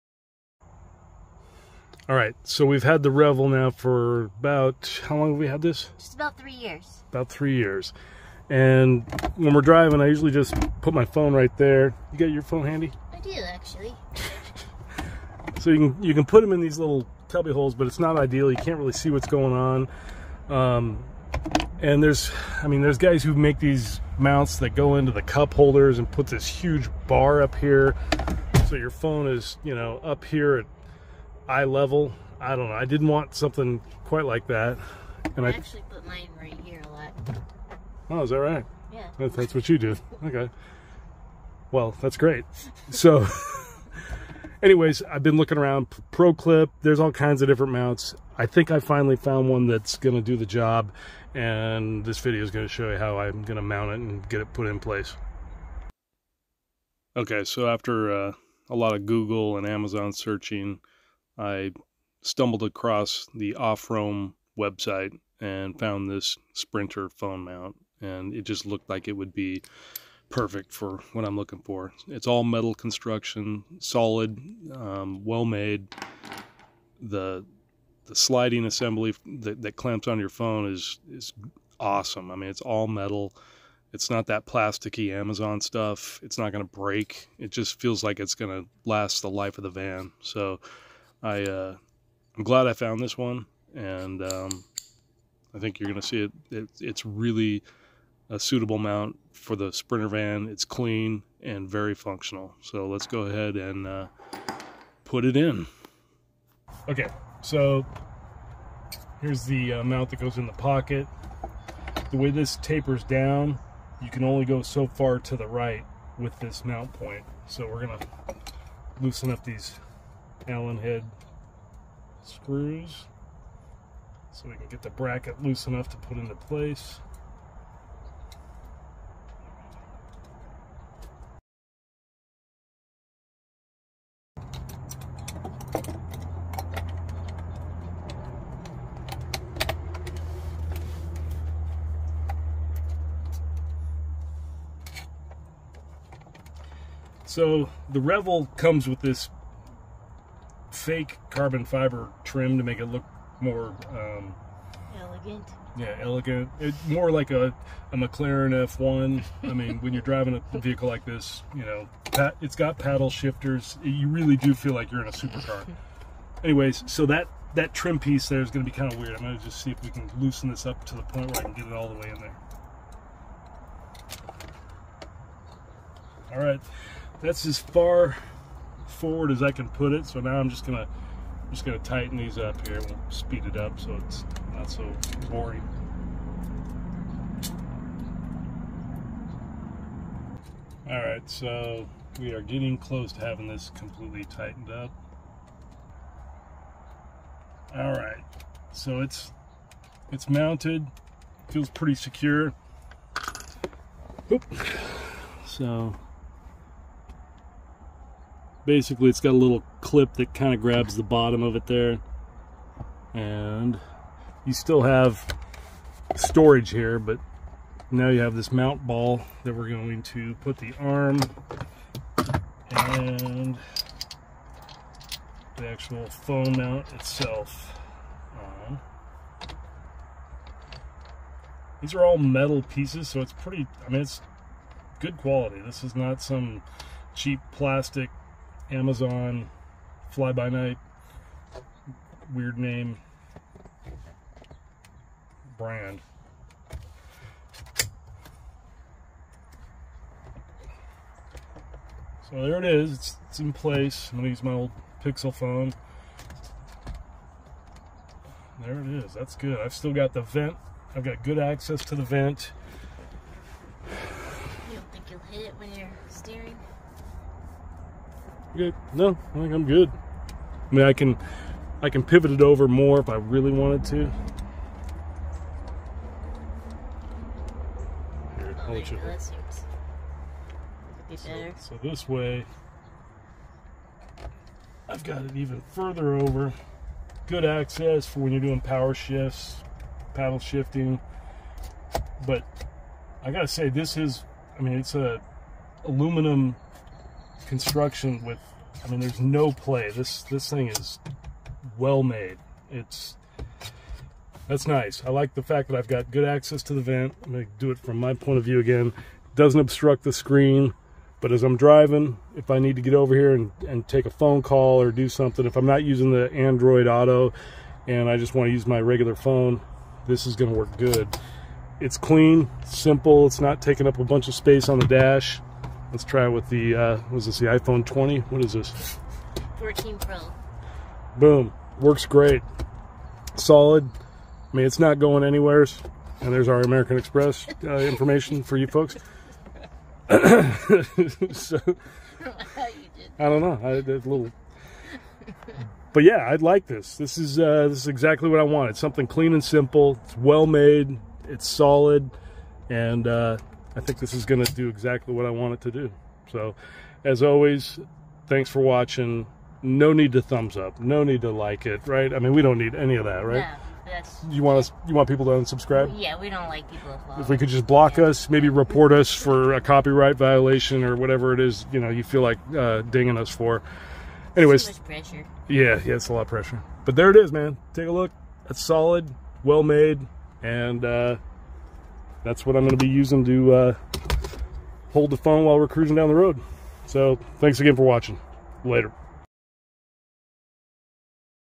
Offroam Phone Mount for the Winnebago Revel / Sprinter Van- SOLID!
I've been looking for a good phone mount for my Revel, not a fan of the plastic cheap ones on Amazon or something that mounts it up high in the driver's field of view above the dashboard. I found this Offroam after a long search and I'm very happy with it- 100% metal construction, extremely solid and holds the phone very securely. This is not a sponsored video, I paid for this unit. If you want one here is a